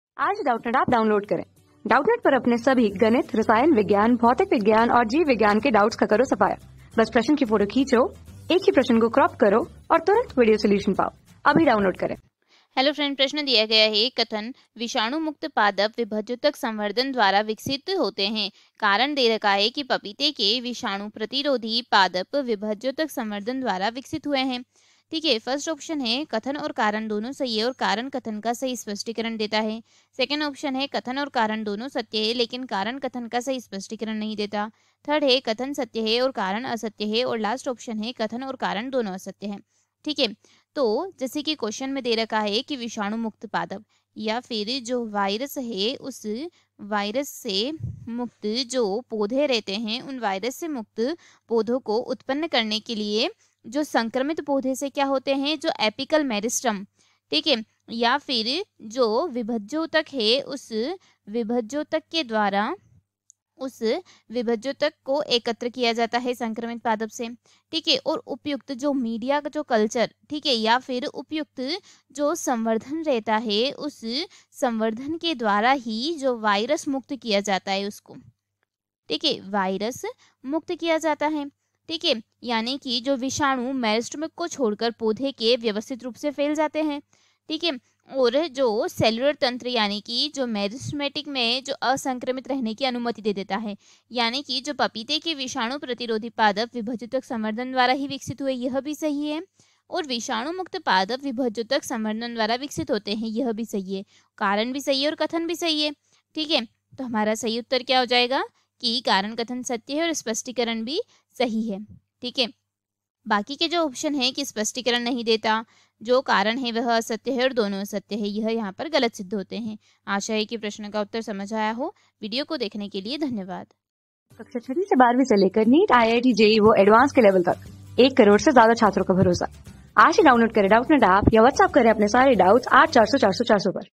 कथन :- विषाणु मुक्त पादप विभज्योतक संवर्धन द्वारा विकसित होते है। कारण :- पपीते के विषाणु प्रतिर...
कथन :- विषाणु मुक्त पादप विभज्योतक संवर्धन द्वारा विकसित होते है। कारण :- पपीते के विषाणु प्रतिरोधी पादप विभज्योतक संवर्धन द्वारा विकसित हुए है।
Class: 12
Subject: BIOLOGY
Chapter: पादप प्रजनन
Board:IIT JEE

👉 Have Any Query? Ask Us.